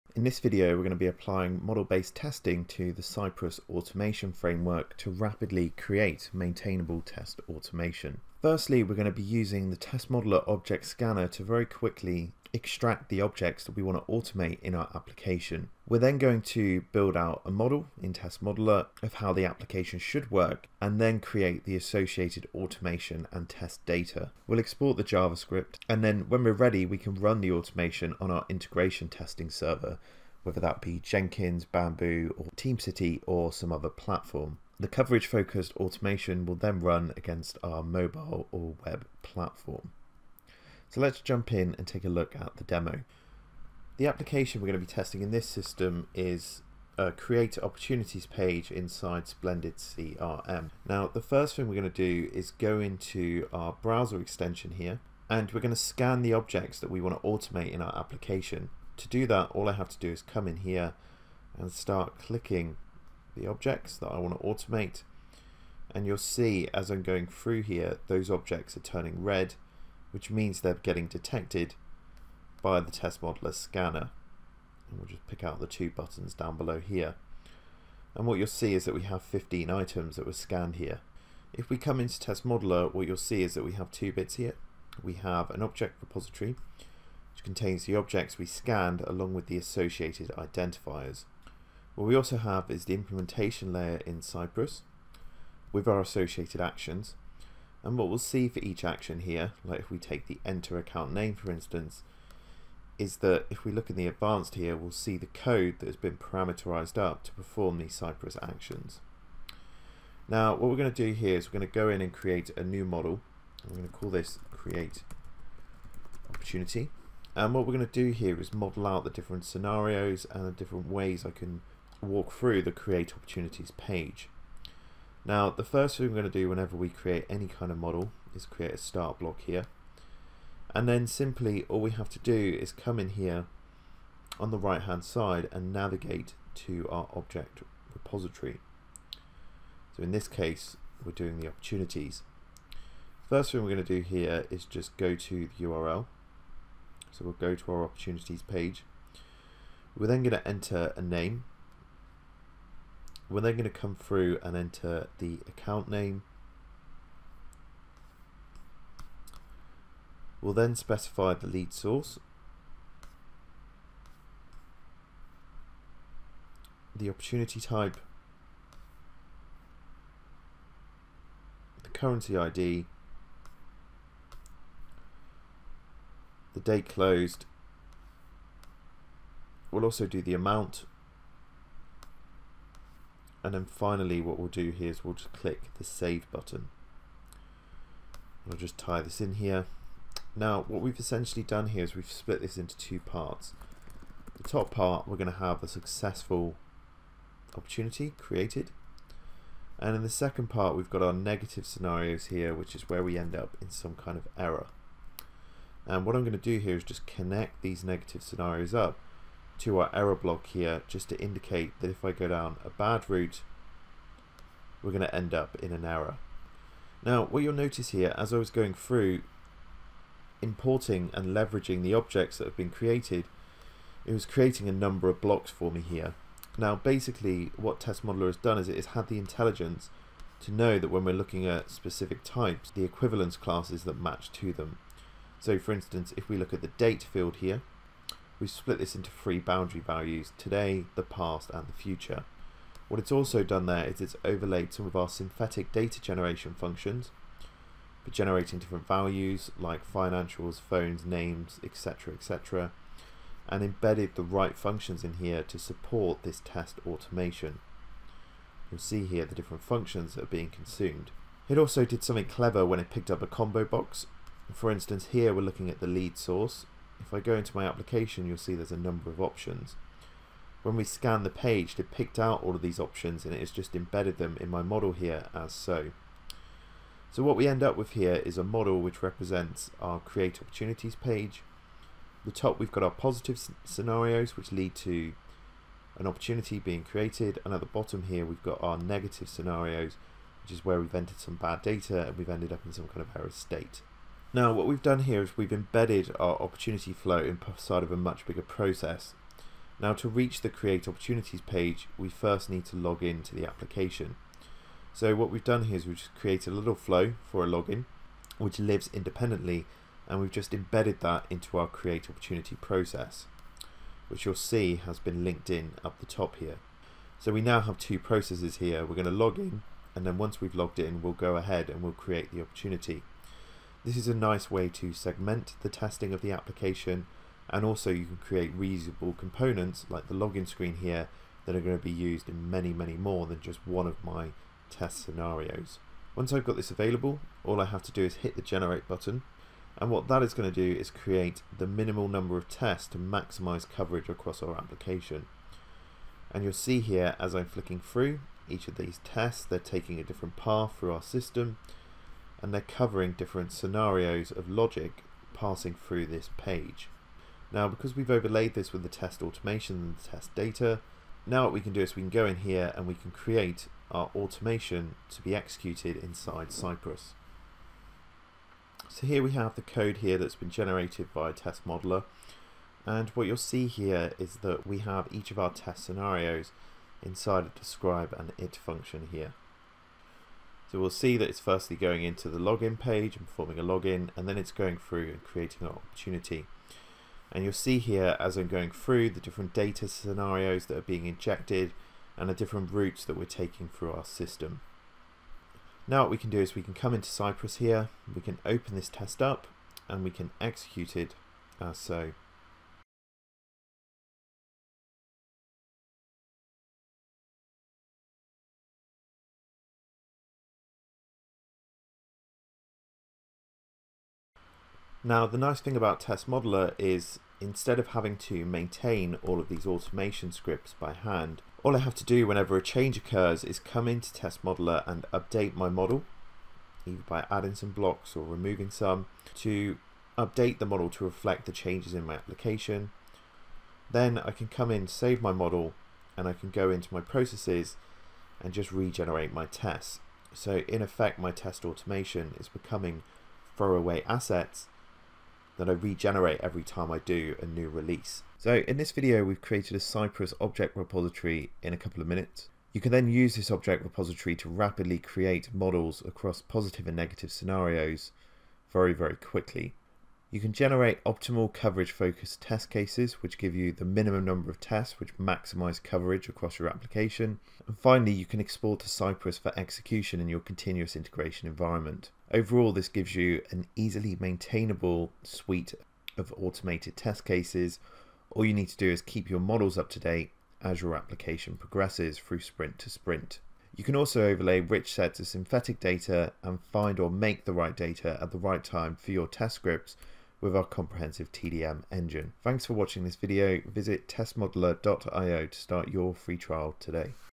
Model-Based Test Automation for Cypress: Rapidly Create and Maintain Rigorous Automated Tests
Follow Curiosity’s socials
Facebook Generate optimised scripts and data for Cypress, rigorously testing fast-changing web applications. Test Modeller removes the bottlenecks associated with manual scripting, data allocation and maintenance, enabling comprehensive testing in short iterations.

Test Modeller allows QA teams and developers to get the most out of Cypress, automatically generating rigorous Javascript tests and data. An application scanner builds Cypress libraries in minutes, and without the need to write copious Javascript. Selecting UI elements generates page objects and the associated implementation, which are then imported to Test Modeller.

Building flowcharts of the web application is then as simple as selecting from the scanned elements. Intelligent modeling identifies the equivalence classes associated with each element, rapidly building models to test positive and negative routes through web applications. Dynamic data definition auto-identifies synthetic test generation functions for each test step, ensuring that every tests comes equipped with valid and up-to-date test data.

Automated test generation creates optimized tests and data, compiling the smallest set of scripts and data needed to test every modelled scenario. As the system changes, testers only need to update the easy-to-maintain flowcharts, generating an up-to-date set of rigorous tests and data.

Watch this short demo of testing a web UI rigorously with a Javascript-based framework, and discover how:
1. An application scanner builds a Cypress repository in minutes, generating executable page objects and a parameterised implementation layer as UI elements are selected.
2. “Fast Modelling” intelligently builds flowcharts from the scanned elements, automatically identifying equivalence classes associated with each imported object.
3. Intelligent data definition tags the model with the data needed to execute every negative and positive scenario, generating complete synthetic test data for every Cypress test.
4. Automated test generation creates the smallest set of Cypress scripts needed to “cover” the model, enabling rigorous testing within short iterations.
5. The compiled code and data can be exported to integration engines like Jenkins, Bamboo and TeamCity, automatically executing rigorous tests within existing CI/CD pipelines.
6. Re-usable subflows enable rapid and rigorous end-to-end testing, assembling blocks visually and applying coverage algorithms to identify paths through complex system logic.
7. Test engineers only need to reflect system changes in the easy-to-maintain models, avoiding the delays created when checking and updating existing automated tests.